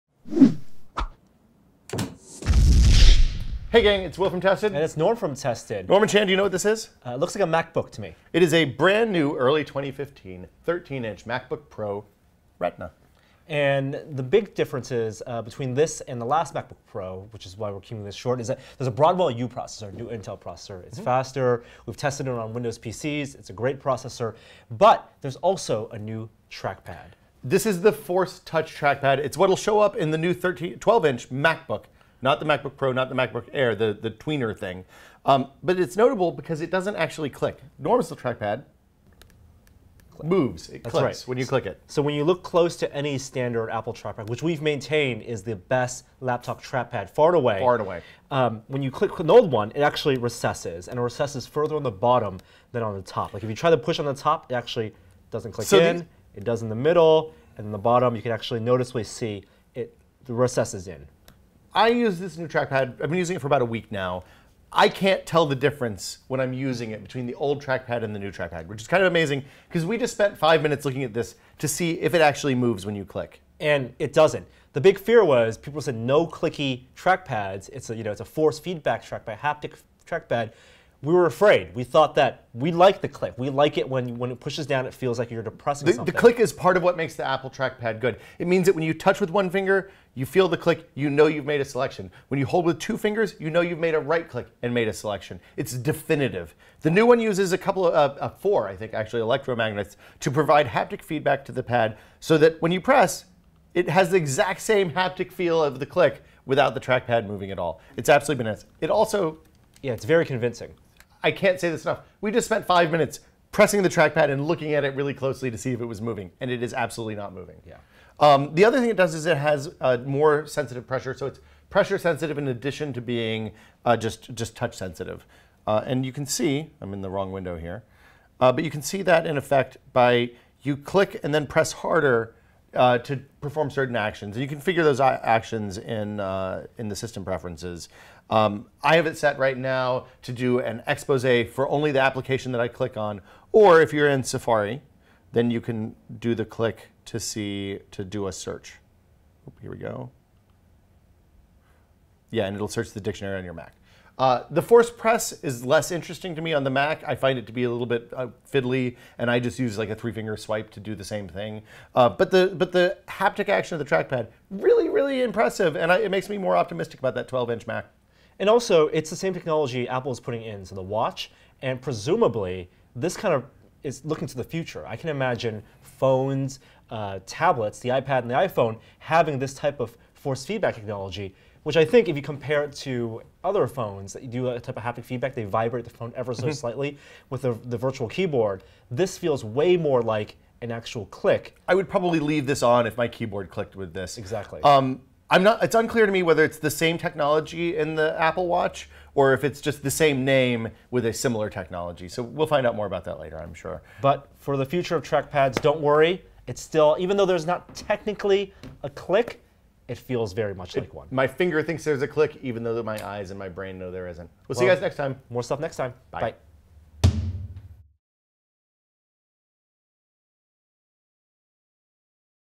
Testing the New Apple Force Touch Trackpad
Apple recently updated its MacBook Pro and MacBook Air laptops with a new trackpad system it's calling Force Touch. We've always maintained that Apple makes the best trackpads, but we were concerned about losing the ability to "click". So here's our test of the new Force Touch system, comparing it to the previous trackpad.